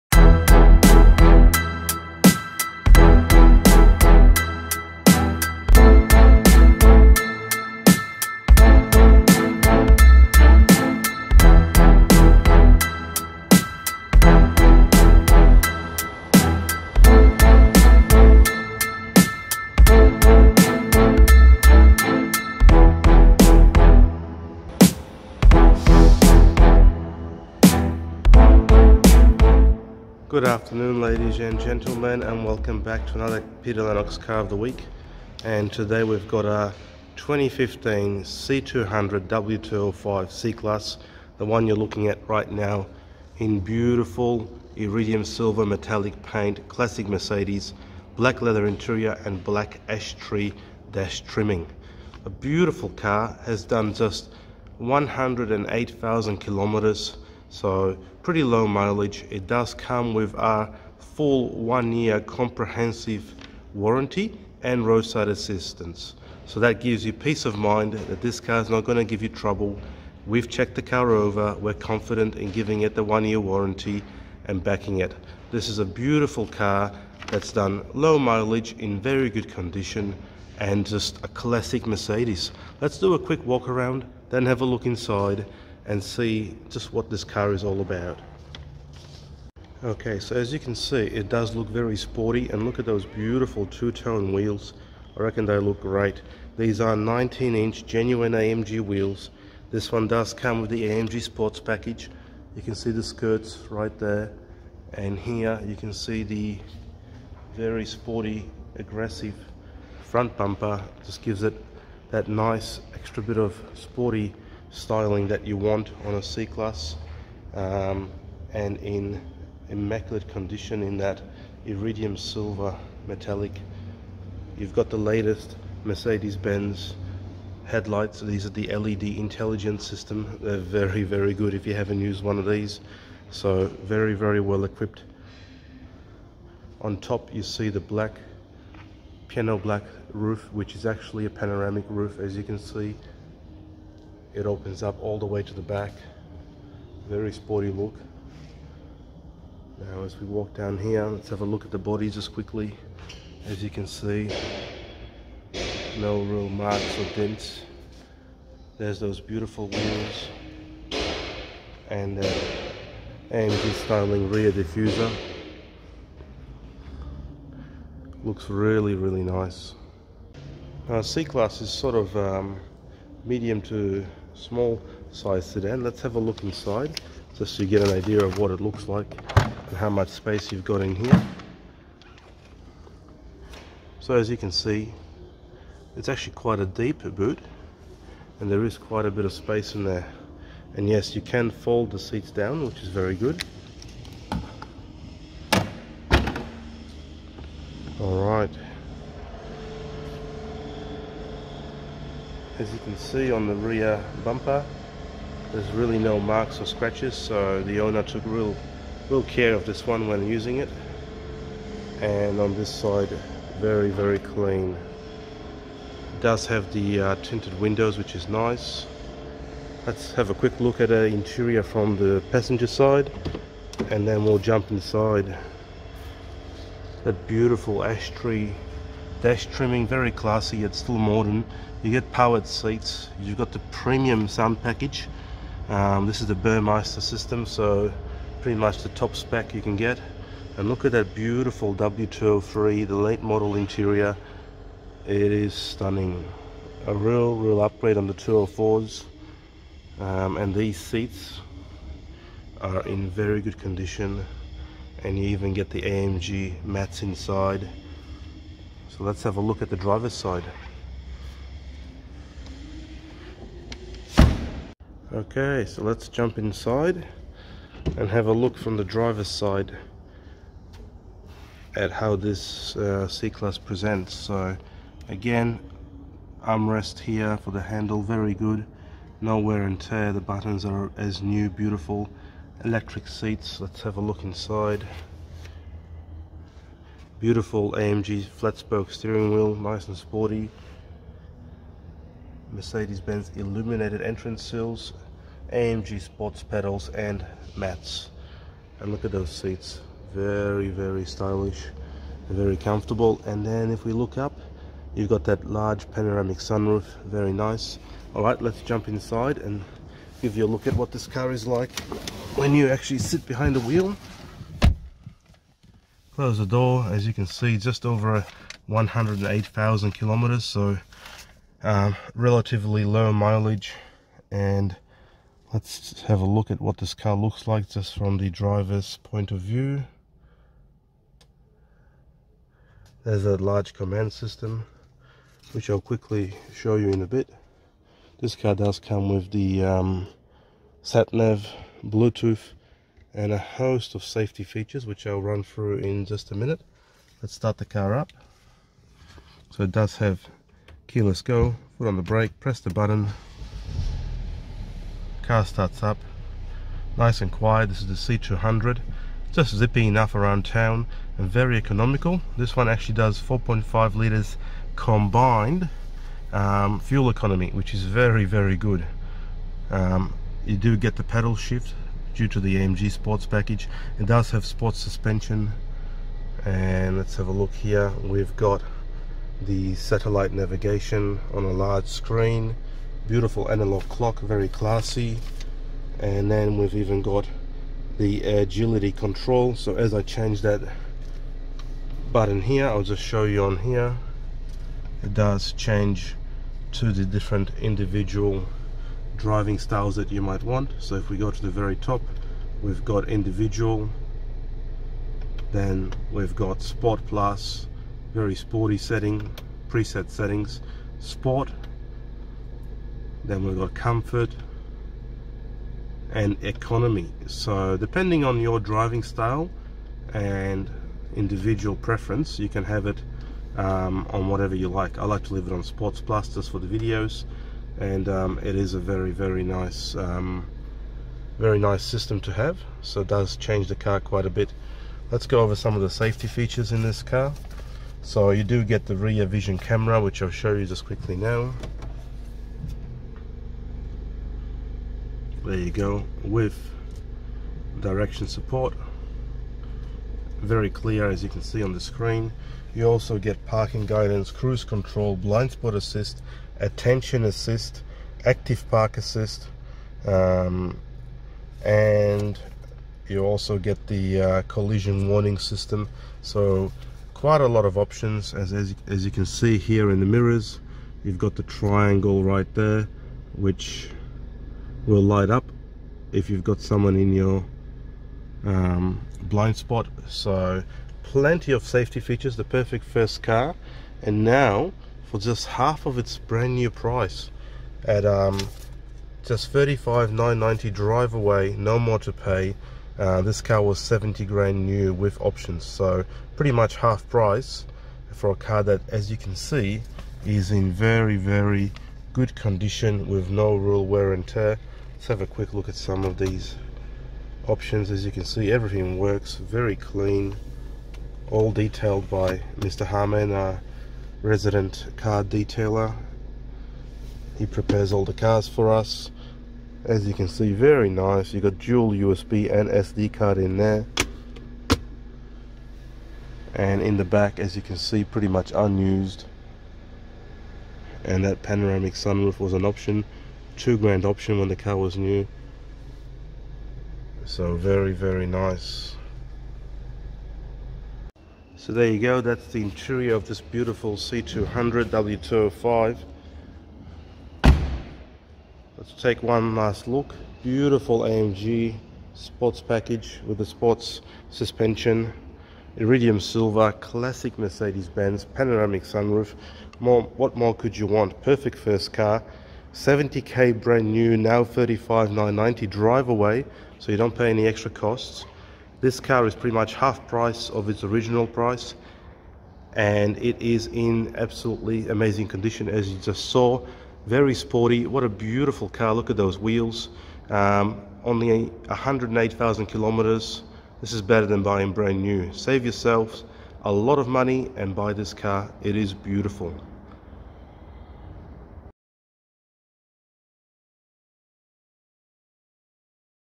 2015 Mercedes C200 W205 C-Class Car of the Week
2015 C200 W205 • JUST $35,990 DRIVEAWAY • ONLY 108,782 KM's • IRIDIUM SILVER • 1 YEAR WARRANTY & ROADSIDE ASSIST!
The Mercedes-Benz C-Class is the perfect car for everyone. It's just the right size, incredibly classy, very luxurious, and one of the most affordable entries into the prestigious Mercedes club!

This C200 is no exception to the above, with its Iridium Silver Metallic, a classic Mercedes colour that demands a second look. It's been well optioned by the original owner, too, with a Panoramic Sunroof, Sports Suspension and that eye-catching AMG Sports Package with two-tone 19" AMG Wheels!

With just under 110K on the clock, this one has been maintained in exceptional condition by its original owners and can now be yours for less than half its new price just six years ago!

Highlights:
- Free 1 Yr Warranty with RSA
- Panoramic sunroof
- Sports suspension
- AMG Styling package
- AMG Line Interior/Exterior sports package
- AMG 19" Alloy wheels
- AMG Floormats
- Service booklet & 2 keys

This C200 is finished in Iridium Silver Metallic paint with Black Leather interior and Black Ash Tree wood trim pieces. It has such features as a rear camera, parking guidance, cruise control, blind spot, attention assist, active park assist. Electric front seats, full command with sat/nav, Bluetooth phone/audio, digital radio, premium sound system, heads up display, A/C Cooling power package, keyless start, folding mirrors, collision warning system and much more!

Step into luxury with the finest features and quality that only Mercedes can bring to the table. This classy C-Class is now on offer with our 1 Year Warranty & Roadside assistance, nothing to worry about... enjoy the drive!